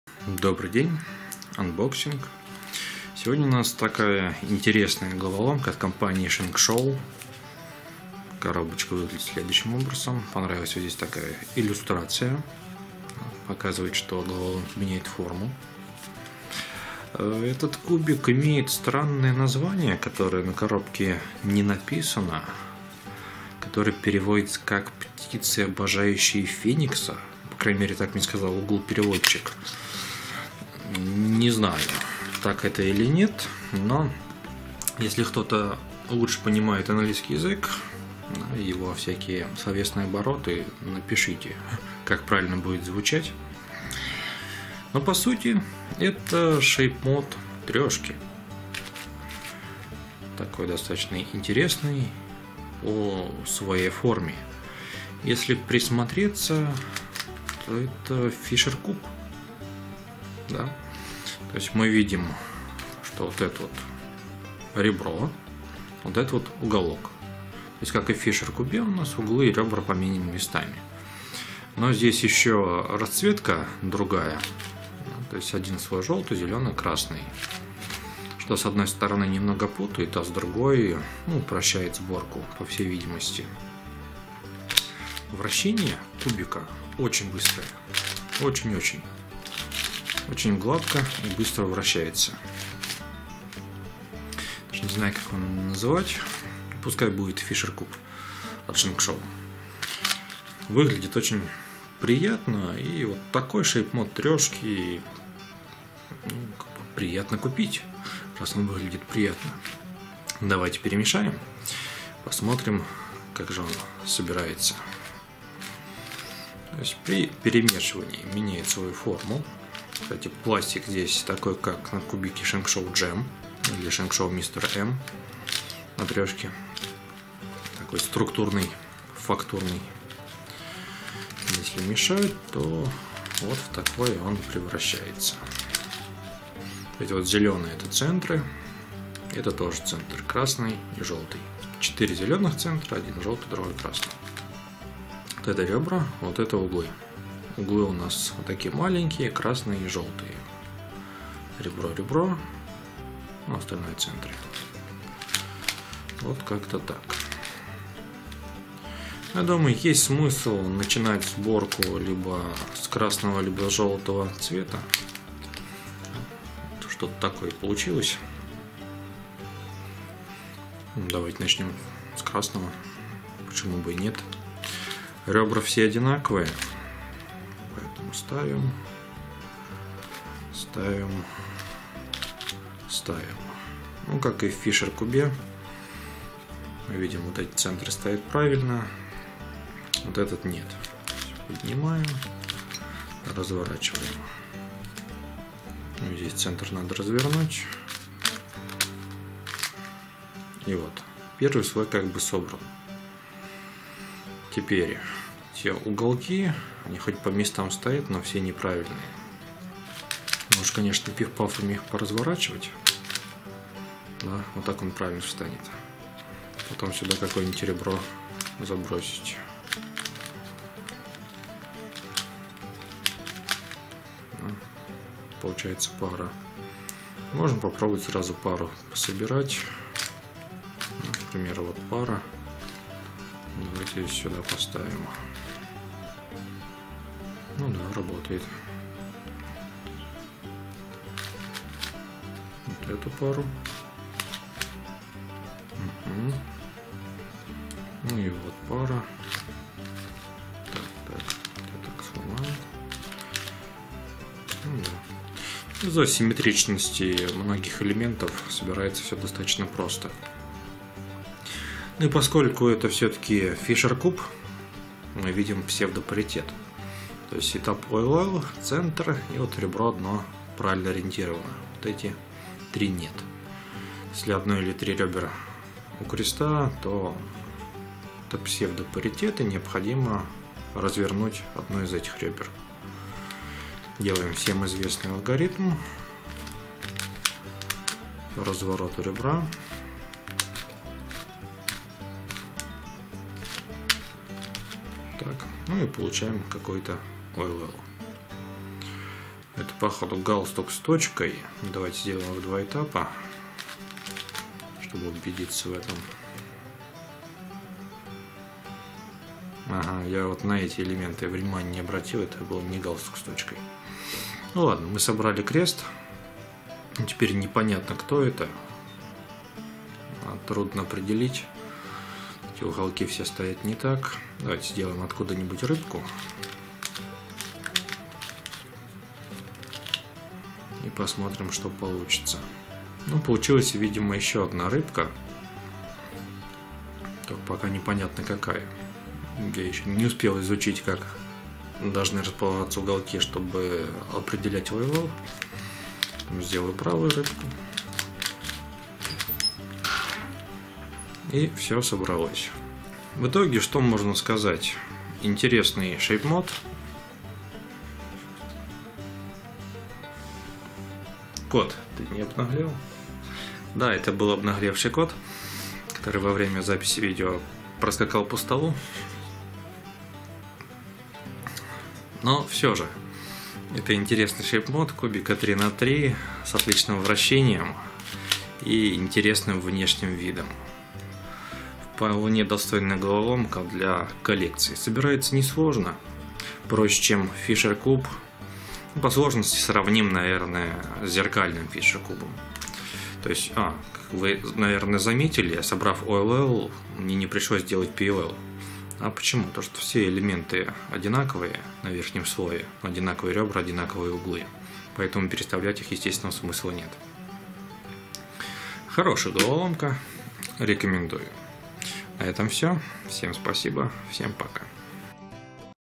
Unboxing №103 ShengShou - Birds Adoring the Phoenix 3x3x3 cube
Приятная во всех отношениях головоломка с отличным вращением. Является шейпмодом обычного кубика Рубика 3х3, а точнее аналогом фишер-куба. Собирается легко, в коллекции будет смотреться хорошо.